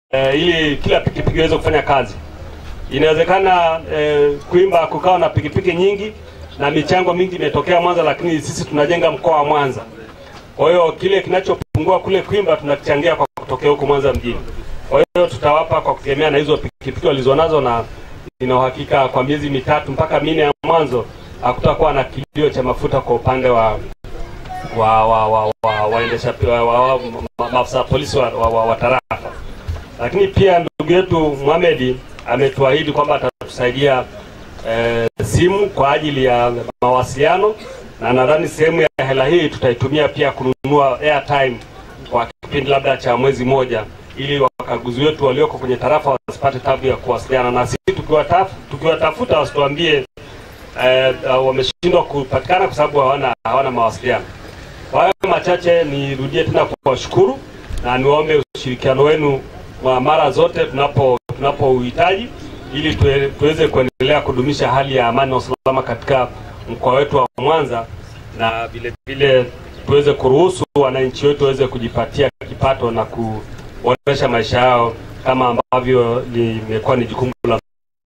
HOTUBA YA R.P.C MWANZA 23/1/2013
Kamanda Mkuu wa Mkoa Afande Ernest Mangu, akihutubia viongozi na wageni waalikwa waliofika katika viwanja vya Mabatini line polisi kwa ajili kushuhudia ugawaji wa mapikipiki katika tarafa zote za Mkoa wa Mwanza kwa ajili ya kufanikisha ulinzi shirikishi. barmedas.tv walifika na kuchukua tukio hilo live